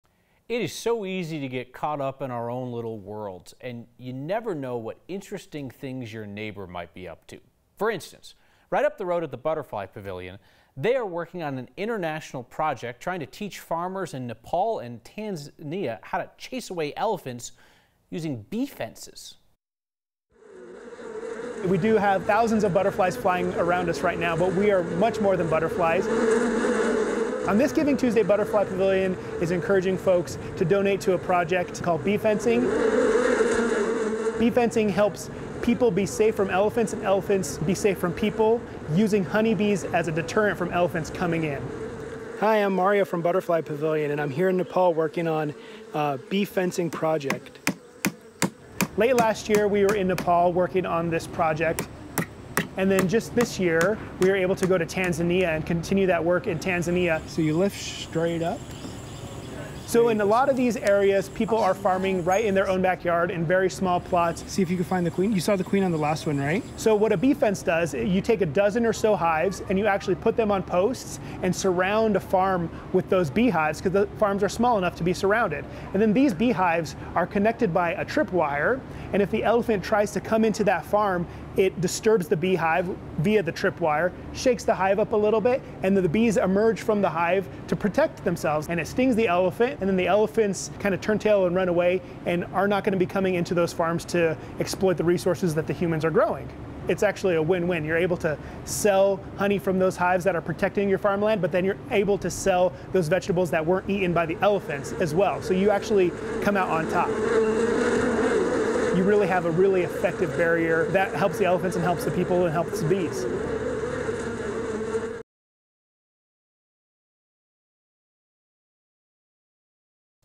How elephants' fear of bees (can't blame them) helps farmers
The Butterfly Pavilion in Westminster, Colorado, is helping teach farmers in Nepal and Tanzania how to repel elephants with 'bee fences.'